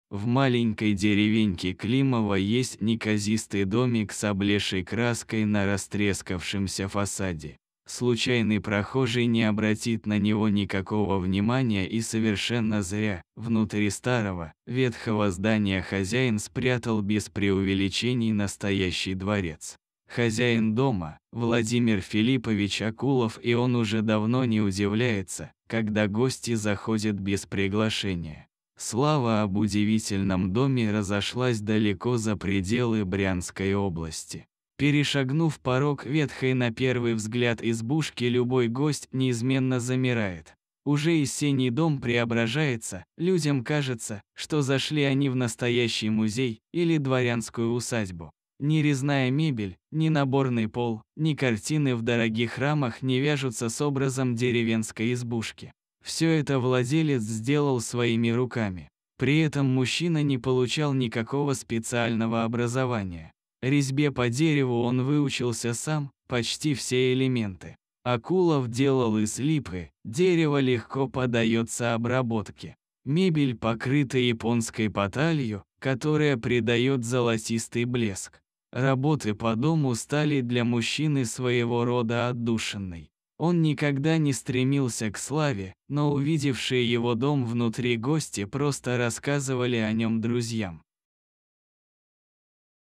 Снаружи неприметный дом, а попадешь внутрь, челюсть отвиснет...
В маленькой деревеньке Климова есть неказистый домик с облезшей краской на растрескавшемся фасаде. Случайный прохожий не обратит на него никакого внимания и совершенно зря: внутри старого, ветхого здания хозяин спрятал без преувеличений настоящий дворец.